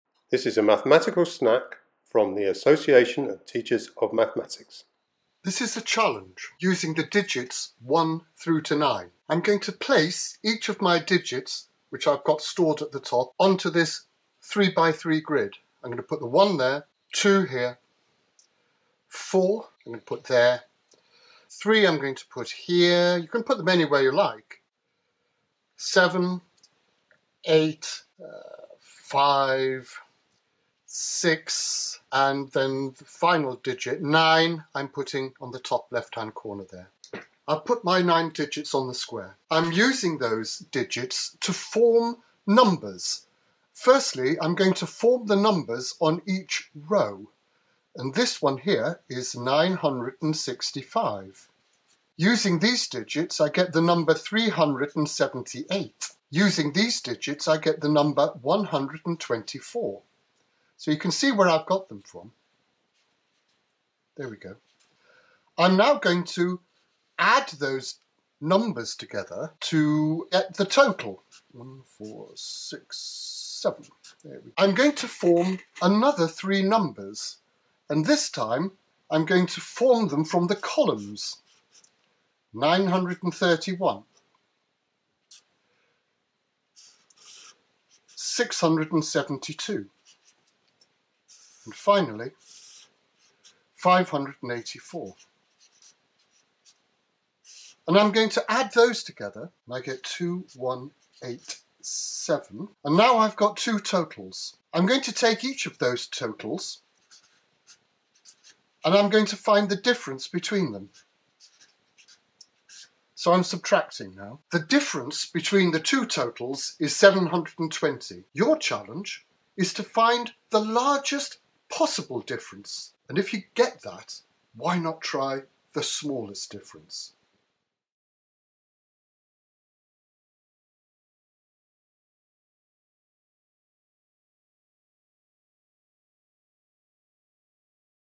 Largest Difference - an ATM 'Maths Snacks' video
'Largest Difference' is a mathematics game, part of the Association of Teachers of Mathematics (ATM) 'Maths Snacks Videos' series - accessible 2 minute videos designed to help develop maths skills and ideas. Suitable game for all ages.
Can you find the largest difference in totals between 2 sides of a 9 square grid?
See all the 'Maths Snacks Videos' on this ATM YouTube channel or on ATM's